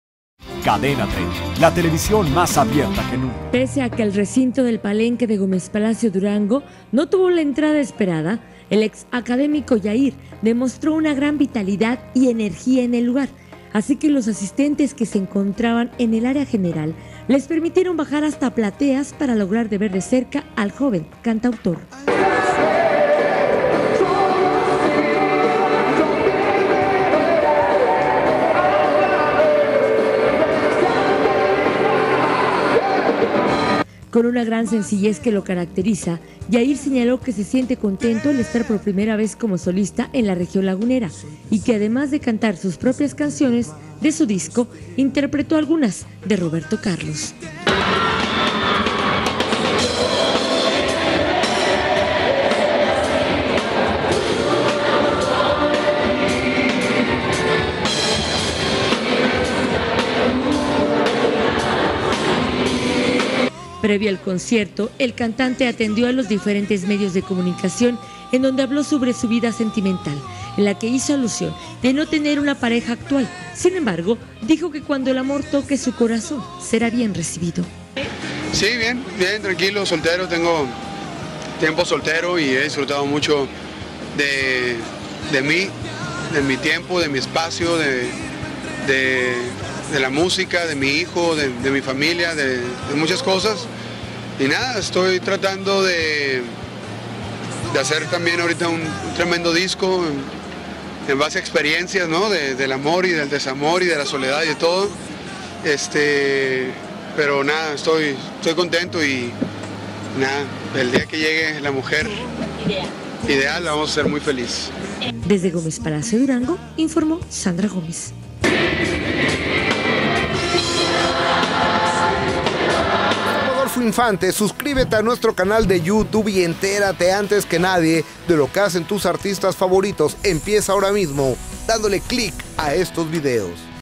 Yahir ofrece concierto en Palenque Gómez Palacio, Durango
01 julio 2014

Yahir se presenta con gran éxito en el Palenque Gómez Palacio, Durango.